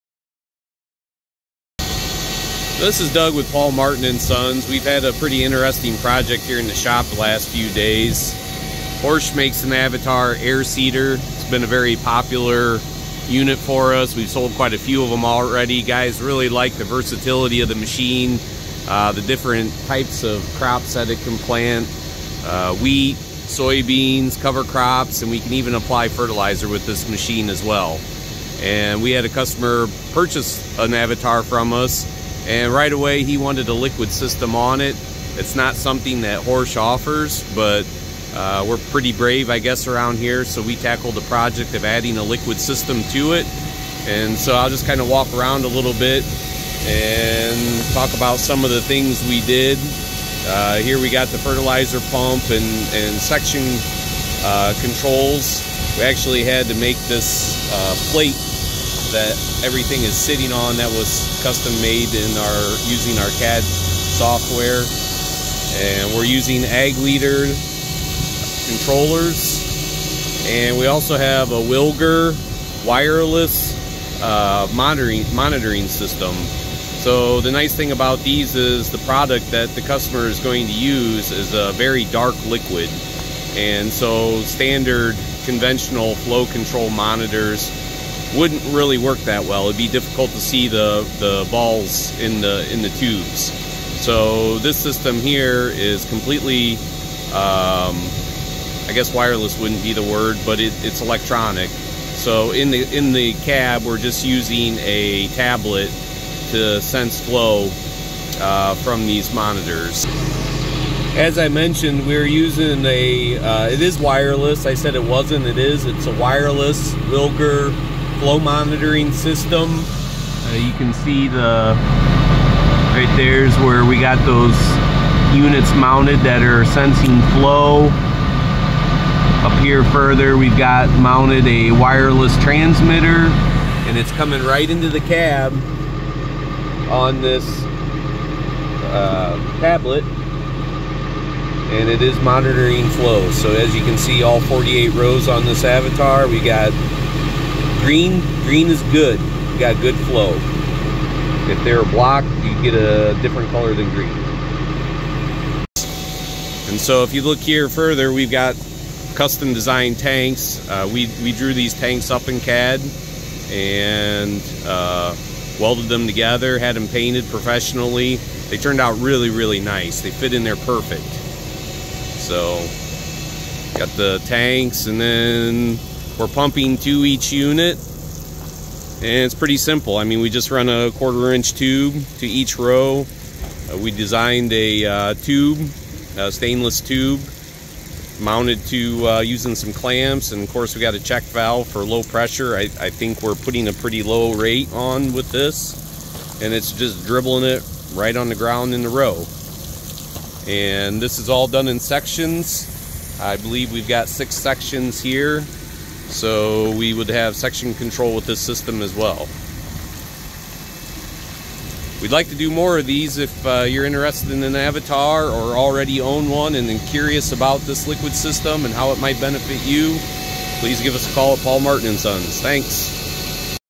Horsch Avatar - New Liquid Fertilizer System
Paul Martin & Sons, located in Napoleon, Ohio, is an agricultural equipment solutions provider selling and servicing Horsch equipment since 2013.  We were recently tasked by customers with designing and installing a liquid fertilizer system for a new Horsch Avatar they purchased.  This system was 100% designed in house by Paul Martin & Sons.